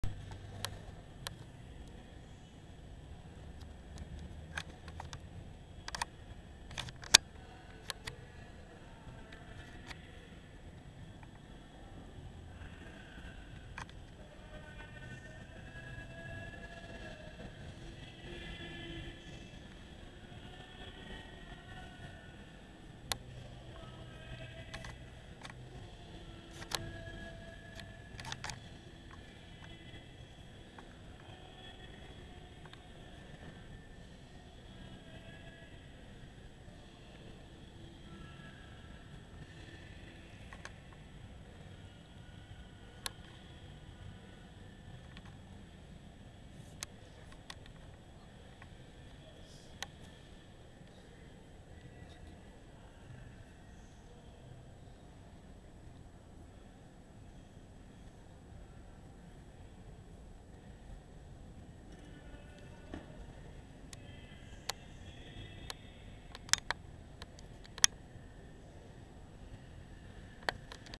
1938 Alfa Romeo 308 en Museo Juan Manuel Fangio(Balcarce Argentina)
Este es un Alfa Romeo de carreras del año 1938 el cual había corrido antes del nacimiento de la F1 y es modelo 308